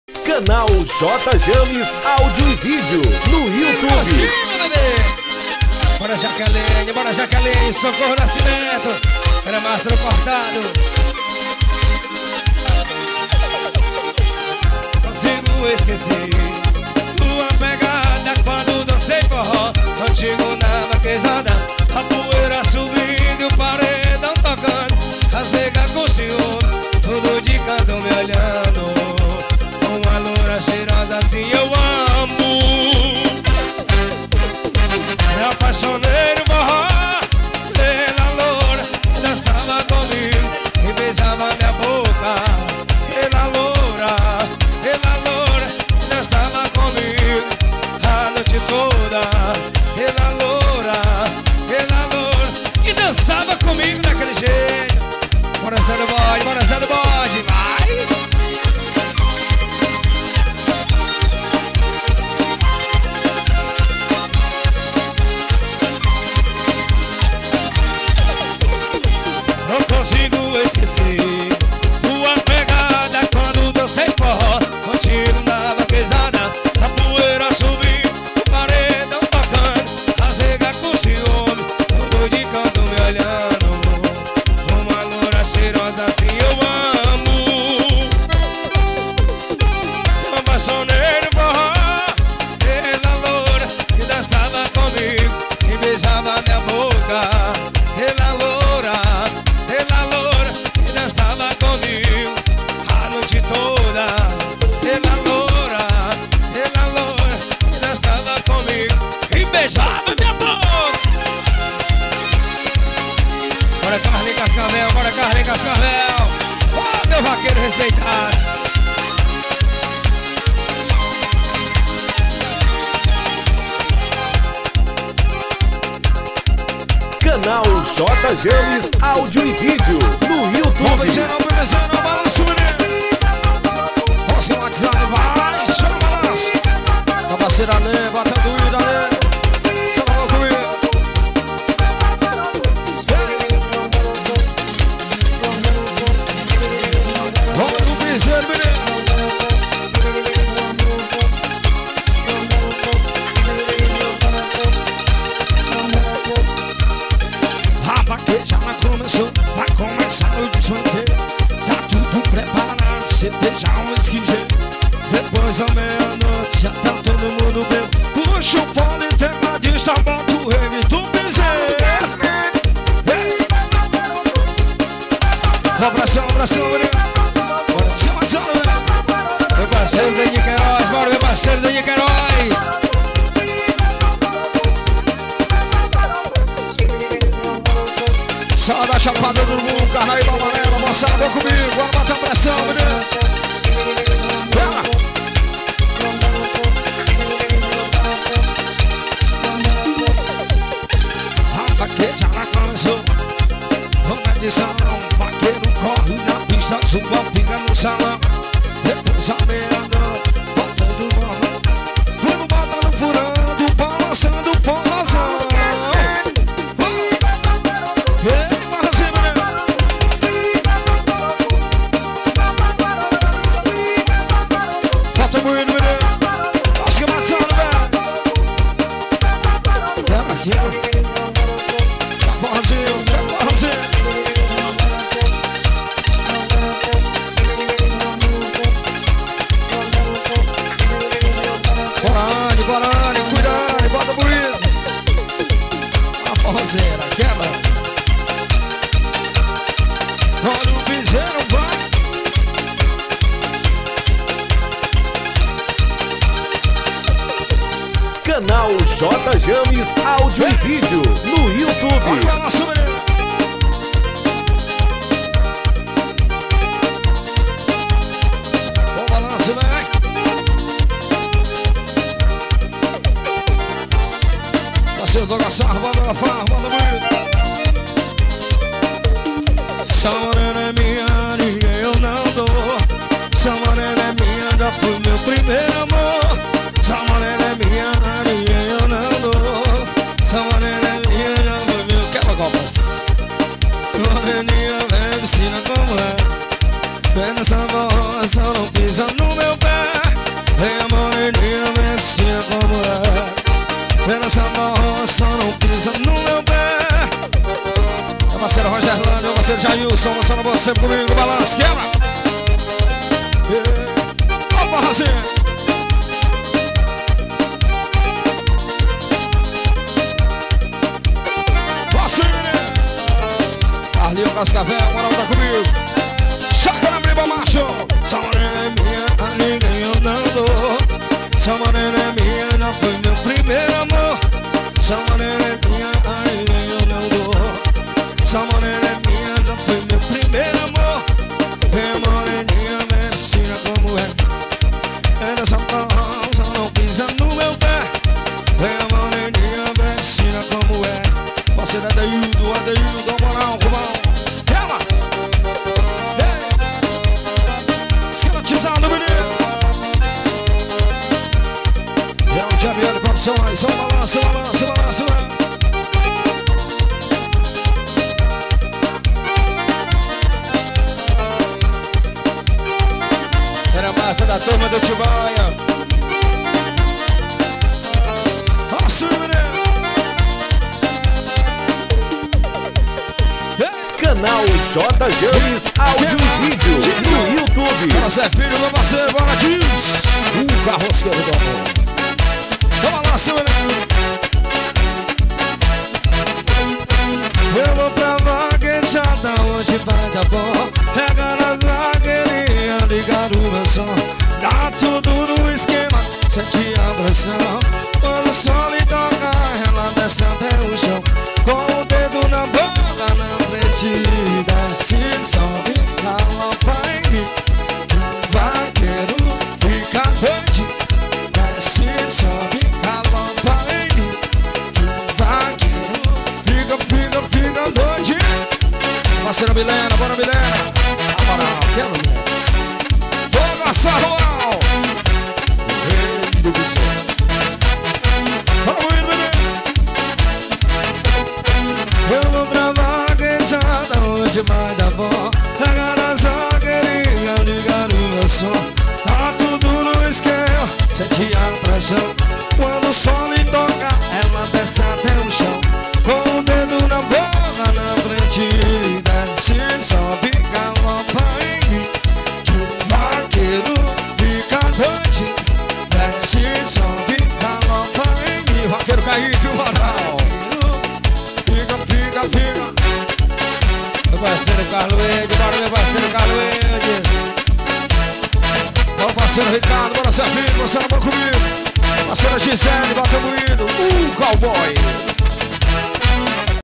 MANELIM E FORRÓ SKEMATIZADO / ARENA BACURIZEIRO - 2024
8ª FESTA   NA ARENA BACURIZEIRO NA COMUNIDADE FREXEIRAS BATALHA PIAUÍ - 2024.
PARTICIPAÇÃO:  MANELIM E FORRÓ SKEMATIZADO.
SONORIZAÇÃO:  PAREDÃO PAQUITO SHOW.
ÁUDIO: GRAVADO AO VIVO DIRETO DA MESA DE SOM EM LR.
IMAGEM E EDIÇÃO:  J. JAMES, E DAVID OLIVEIRA .
ORG:  VAQUEIROS CHICO CABRA VELHO E DENIR.